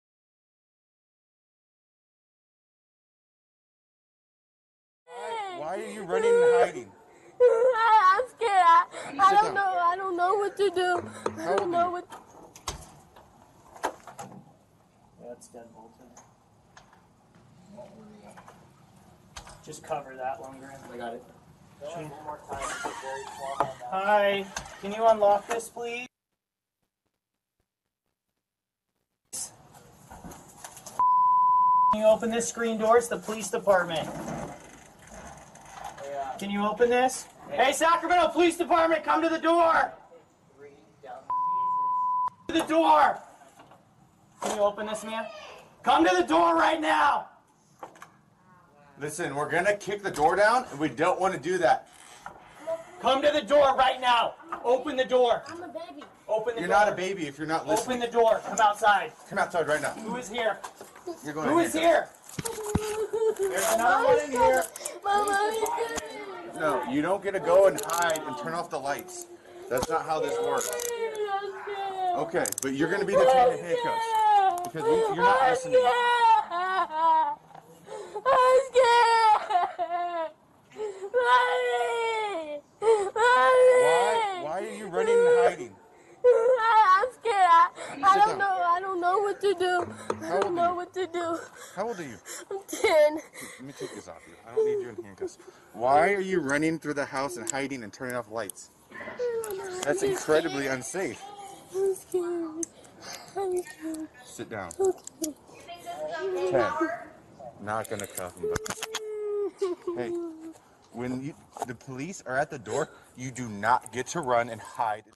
Police handcuff Black girl, 10, wearing pyjamas during raid in Sacramento 1
Sacramento’s police department has come under fire after body camera footage of an officer handcuffing a 10-year-old Black girl was released this week.

The video from February last year was reportedly played at a Sacramento City Council meeting on Tuesday 20 June.

It shows the girl, dressed in red and black pyjamas, being handcuffed while she’s crying, after she opened the door to police officers conducting a probation raid in connection with a stolen car and firearms investigation.

The police account states the investigation involved a “known gang member” who was on probation “and posted both pictures and videos with firearms”.